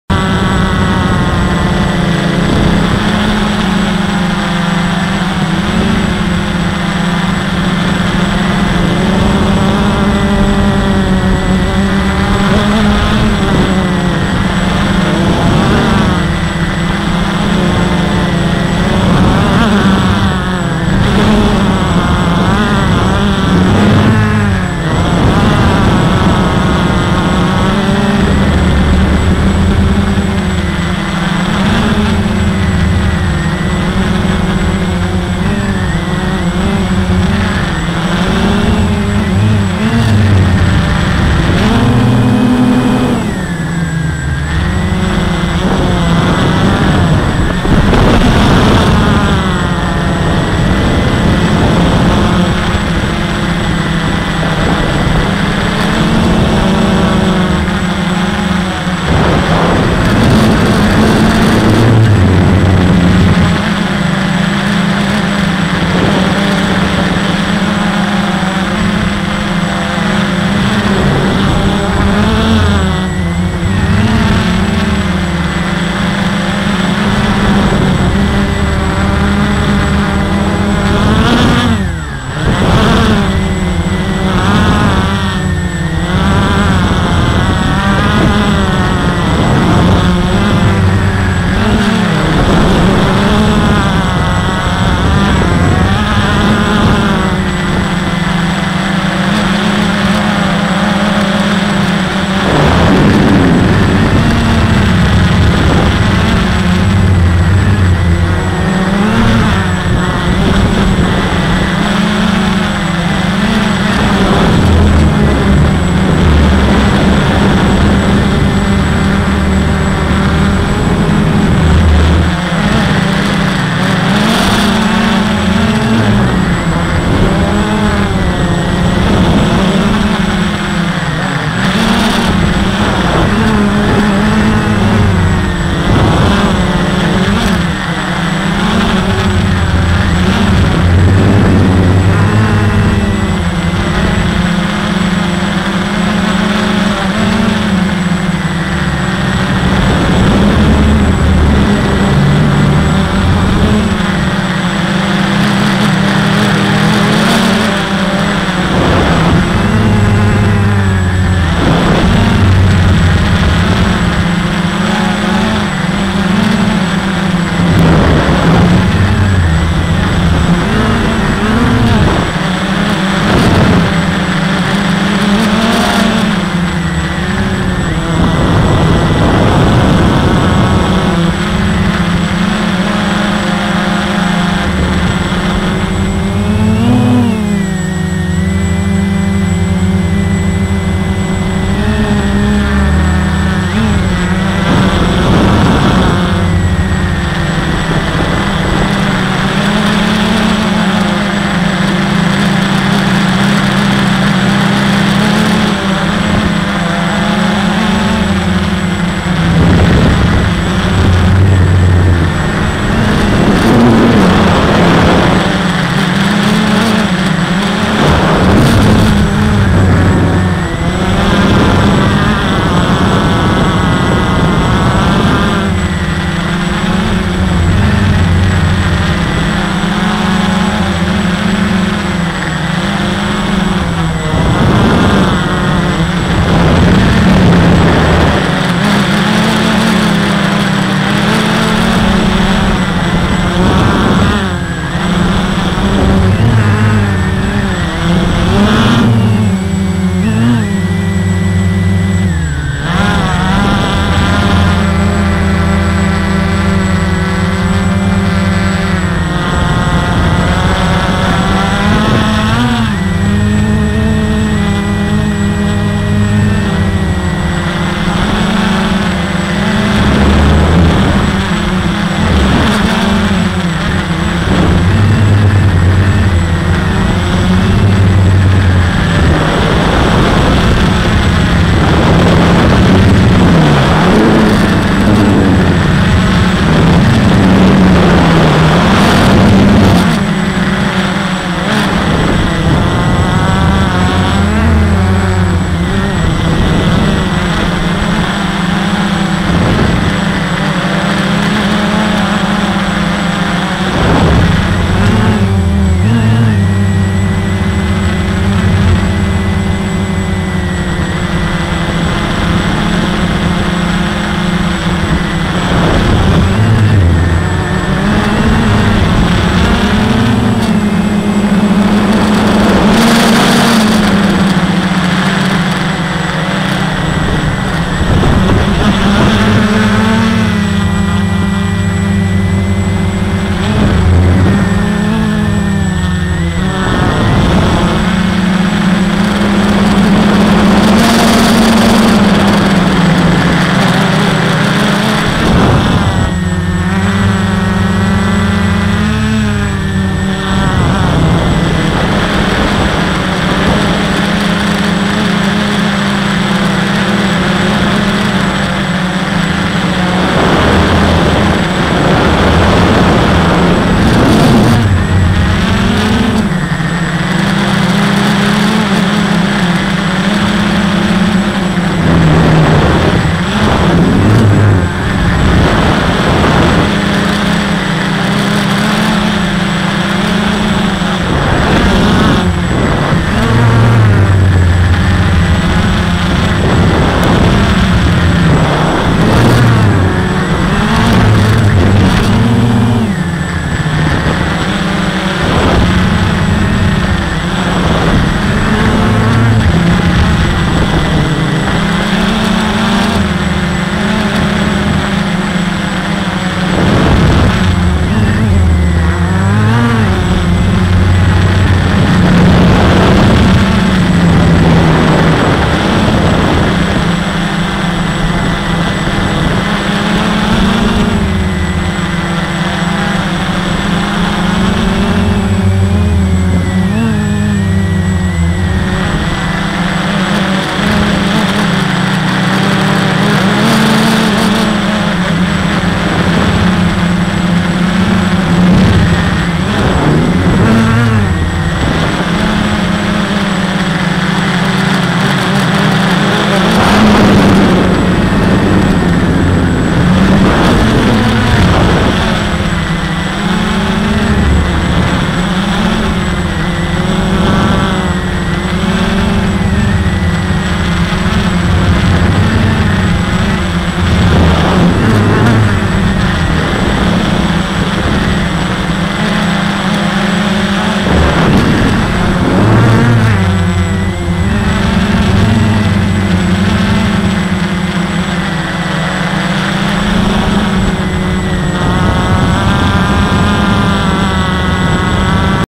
FPV fun - Walkera QR X350
Having fun with my Walkera QR X350. Flying in Manual mode. Also having 2 fpv-passengers in this flight :)